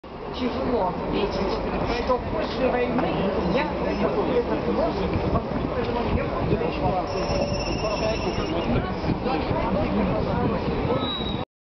Митинг у Юбилейного
Митинг в защиту Петербурга (против газпром сити на Охте) у ст. метро Спортивная